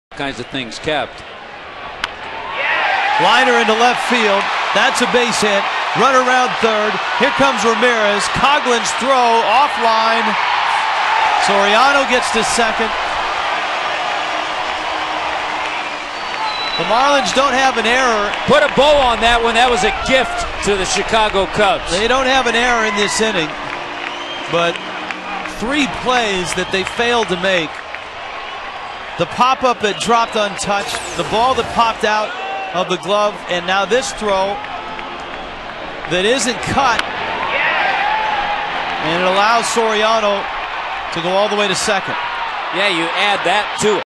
2010/05/10 Soriano's second RBI single
FLA@CHC: Soriano puts Cubs ahead with RBI single